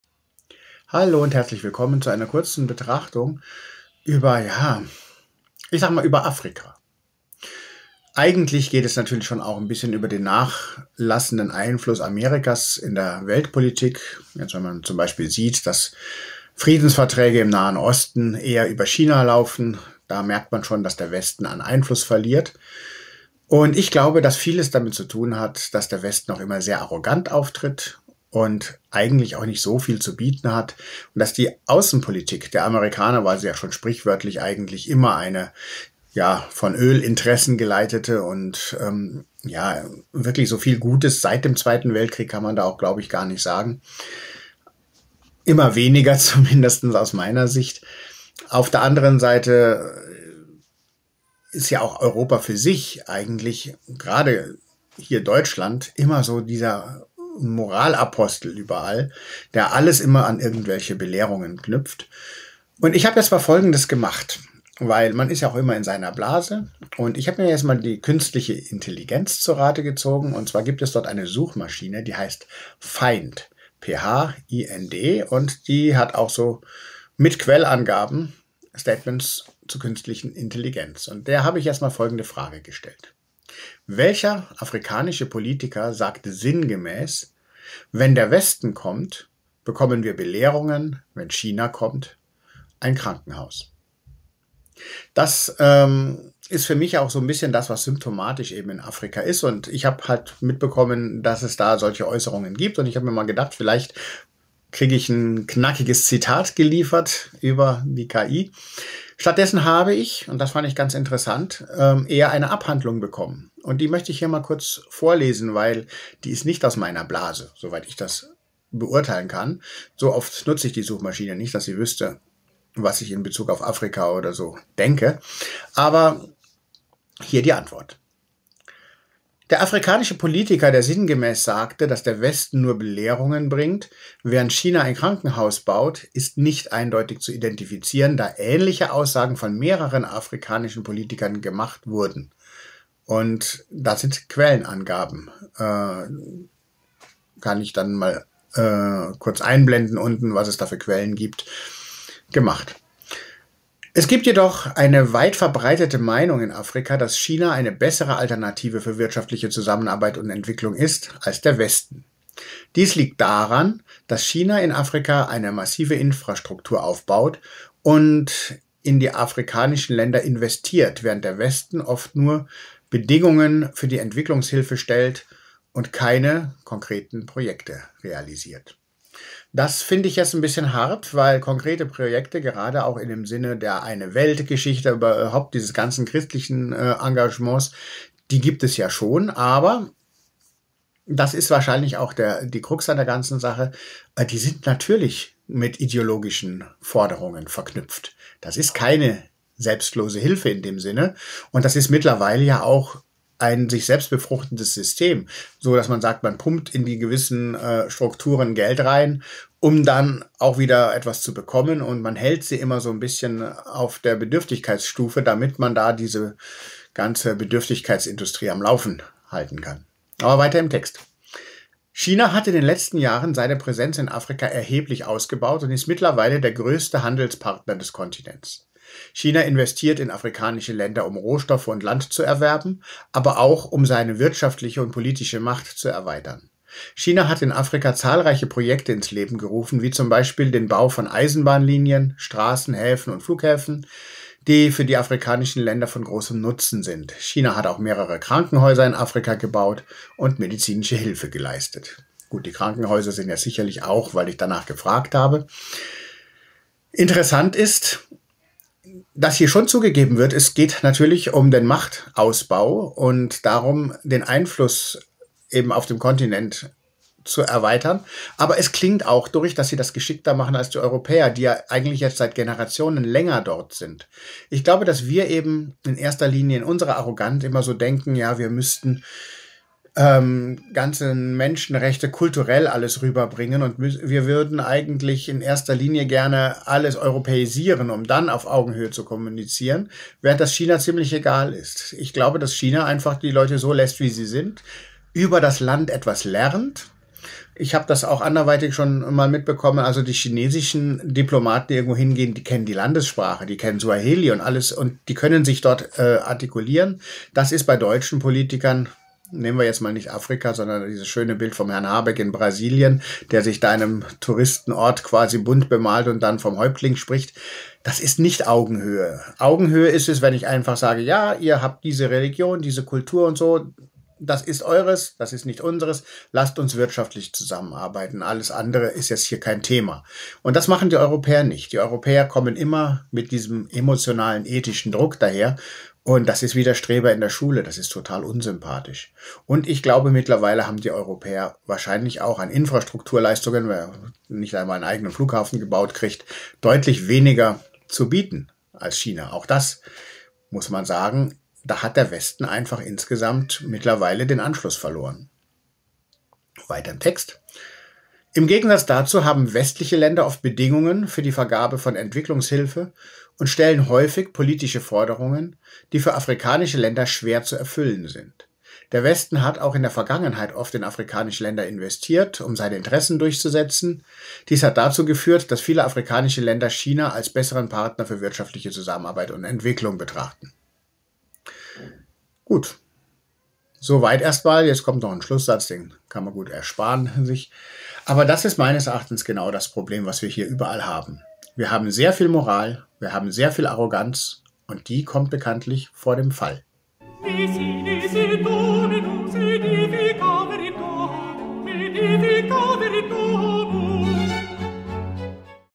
[GP] Afrika, China und Europa.
... und was die KI phind.com dazu zu sagen hat.

Wir schreiben uns gerne die Entwicklungshilfe als Zeichen unserer humanitären Einstellung auf die Fahne. Seltsamerweise punktet China in Afrika mittlerweile deutlich stärker, obwohl es dort mit Humanismus bekanntlich nicht weit her ist. Warum ist das so?